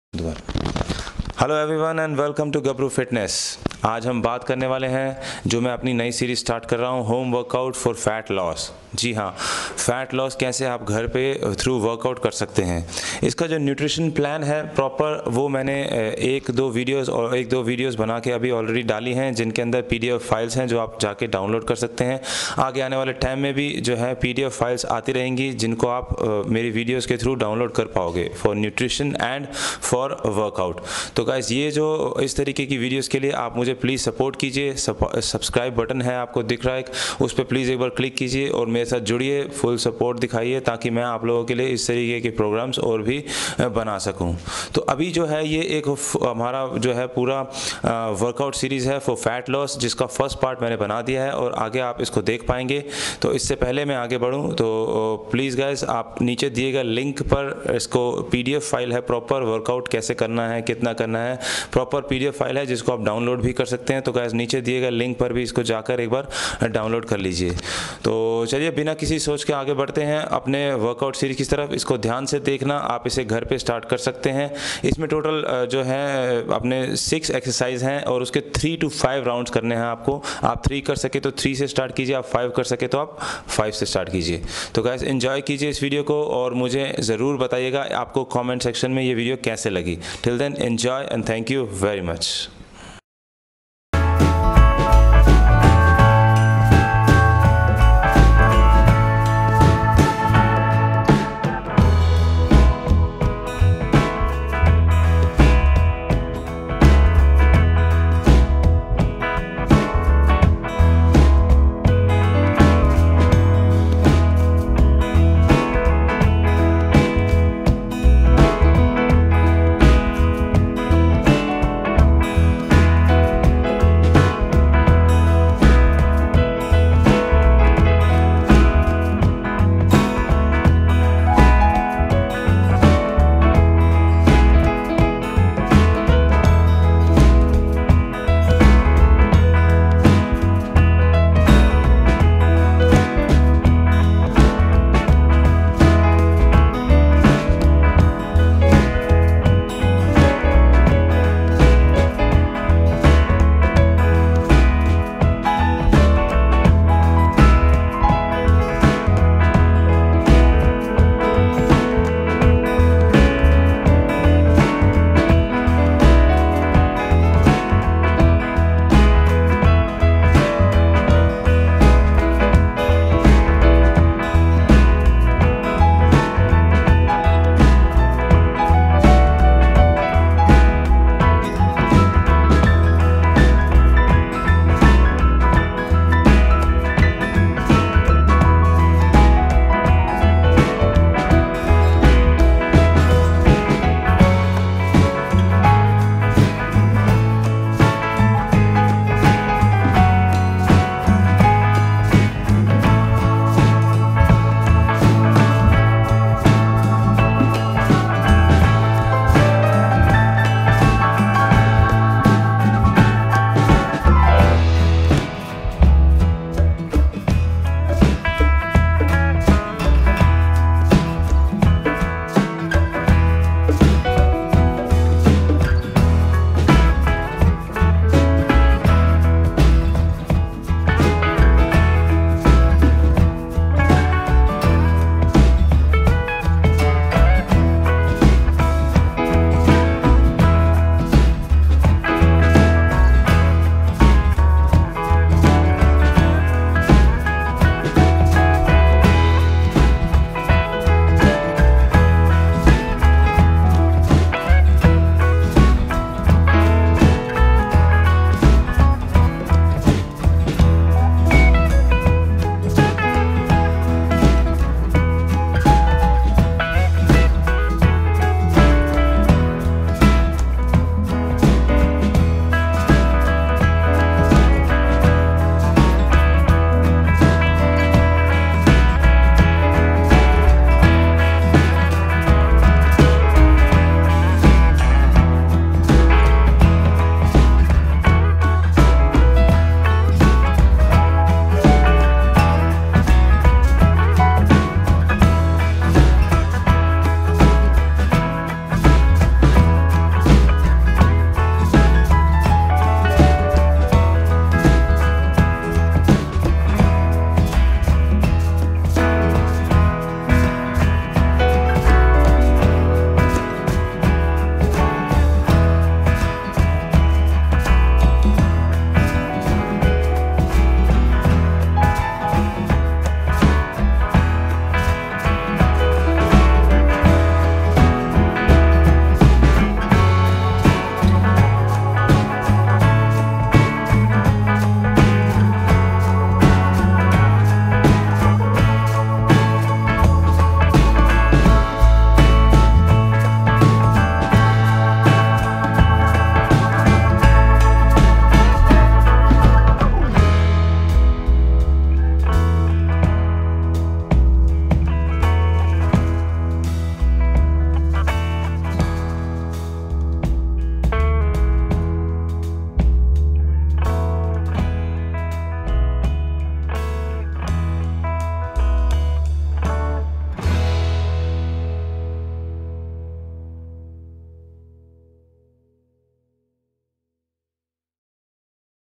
Body Weight - Home Workout for Fat Loss - Part1
Part1 (Follow for 2 weeks and 3 days in a week) of my body weight home workout fat loss series. You have to follow this 12 week program and follow calories deficit diet plan if your goal is fat loss.

Other Imp Links to follow

Full Day Nutrition Plan (Fat loss or Gain Weight)

Gabru Fitness Lifestyle Store